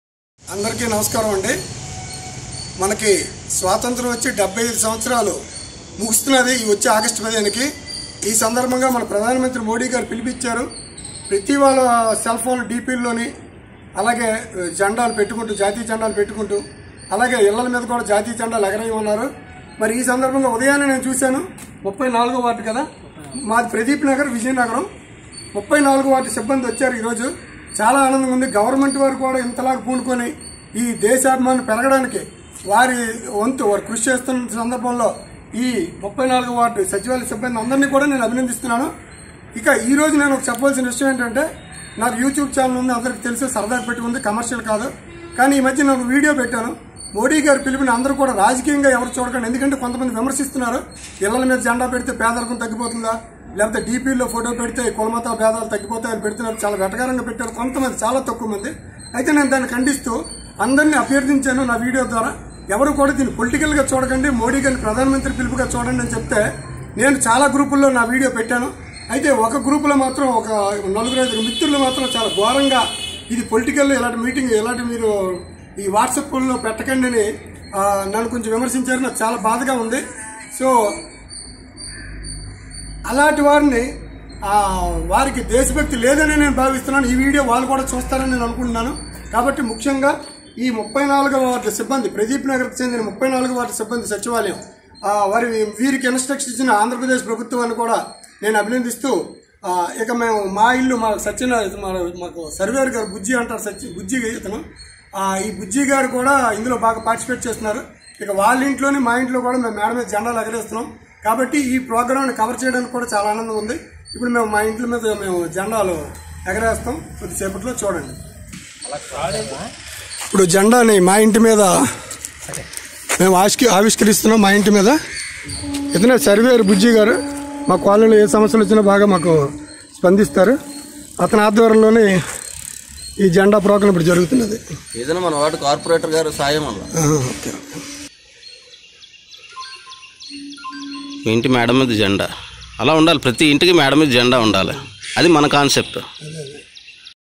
ఇంటింట జాతీయ జెండా కార్యక్రమంలో చురుకుగా పాల్గొంటున్న విజయనగరం సచివాలయం సిబ్బంది 💐💐💐
ఇంటింట జాతీయ జెండా కార్యక్రమంలో చురుకుగా పాల్గొంటున్న విజయనగరం 34 వ  డివిజన్ సచివాలయం సిబ్బంది 💐💐💐